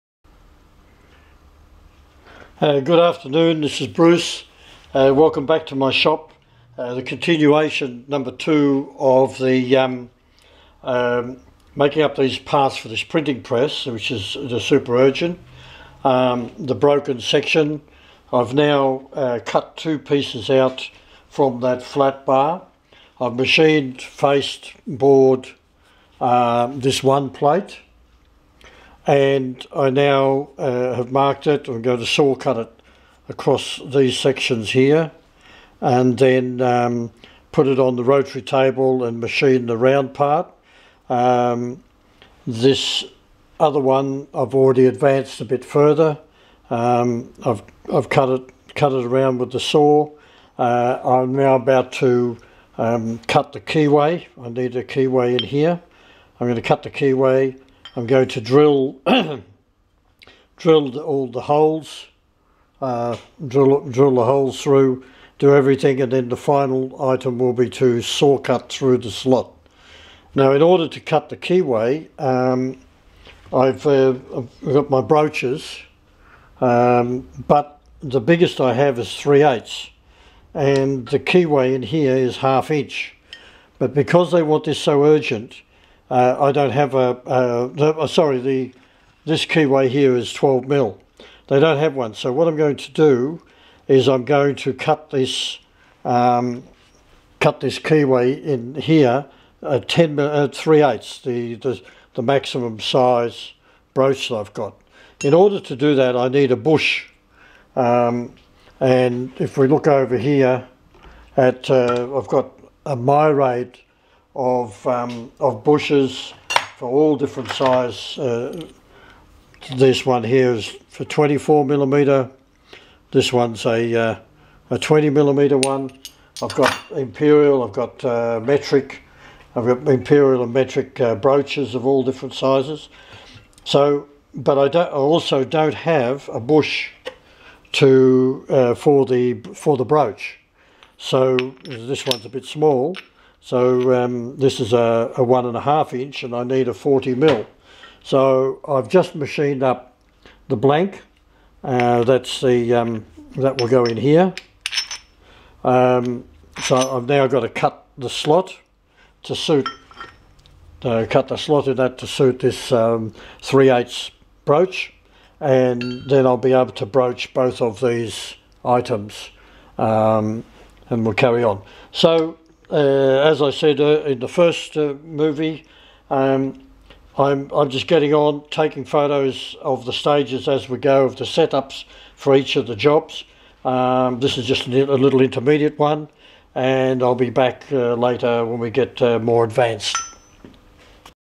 Weekend Special Printing Press 2 oscillating shaft casting replacement
Weekend Special Printing Press 2 Continuing on with the steel build of the printer showing issue faced to cut the keyways, and some progress